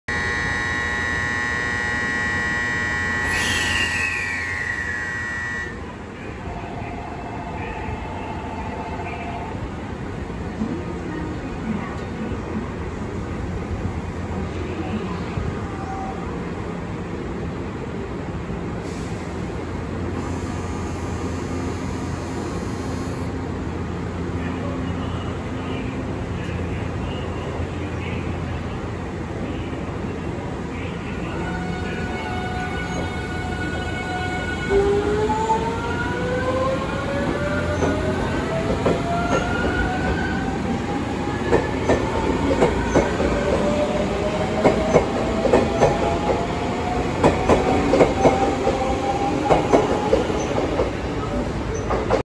JR383系 A8 名古屋工場出場試運転 名古屋駅発車
新幹線で名古屋に降り立ち、
在来線ホームでカミンズの音につられて11番線にやってきたこの人。
キハ75やキハ11の排気を浴びしばらくした頃。
するとホームの先端に数人撮り鉄がいるではないですか。

383系の試運転がやってきました。
6両のA8編成です。